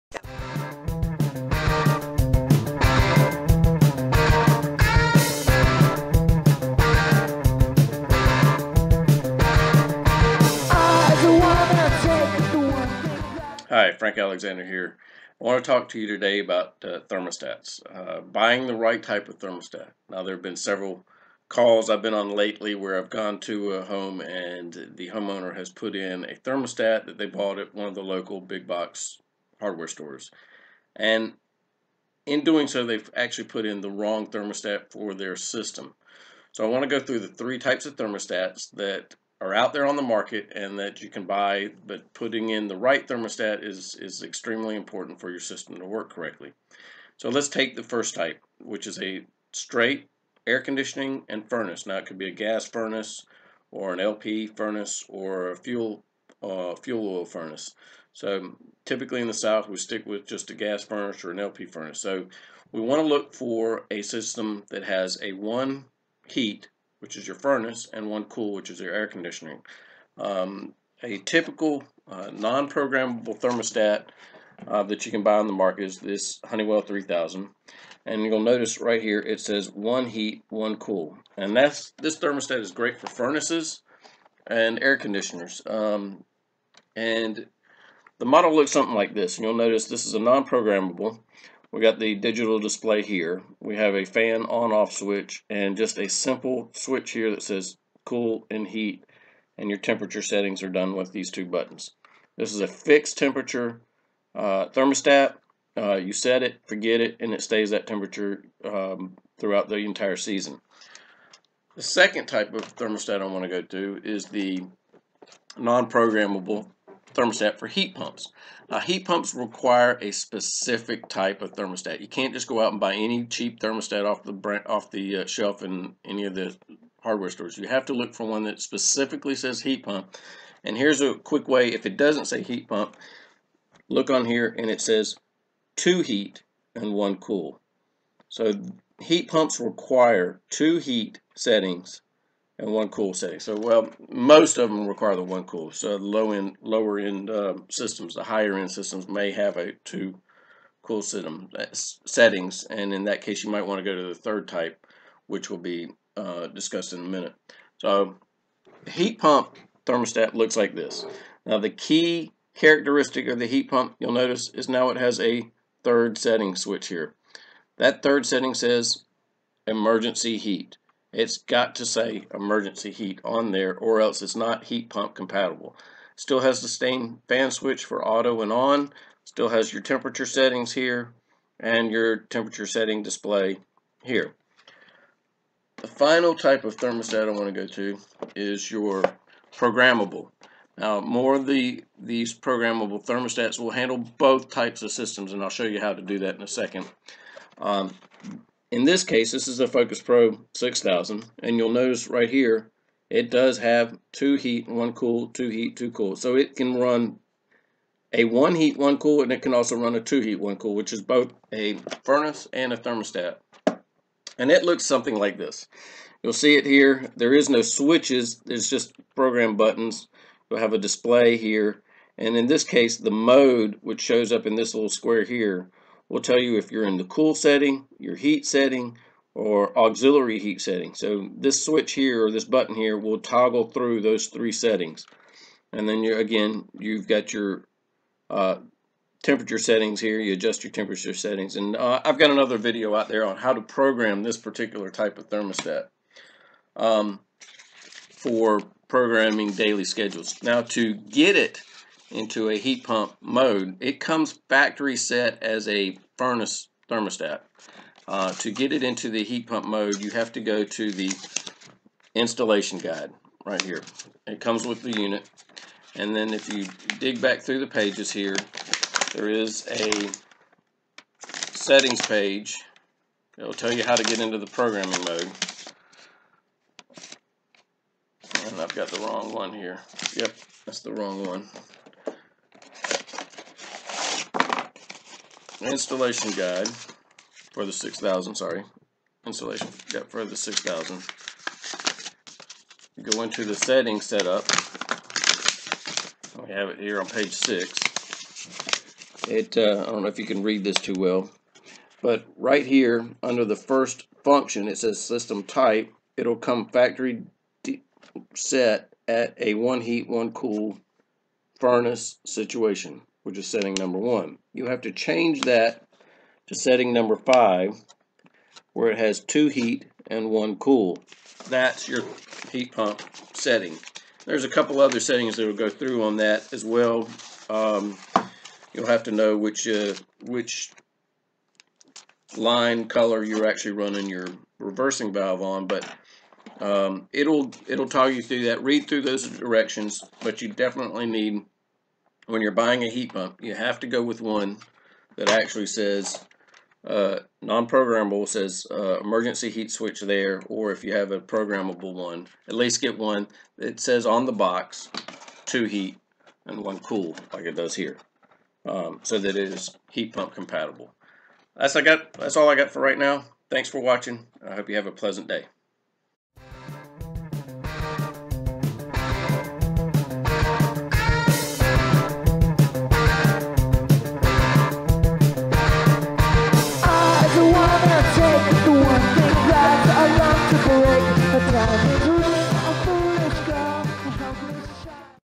Selecting the Right Thermostat
Which thermostat is right for me?
There are three basic types of thermostats, choosing the right one will save you lots of money and headaches.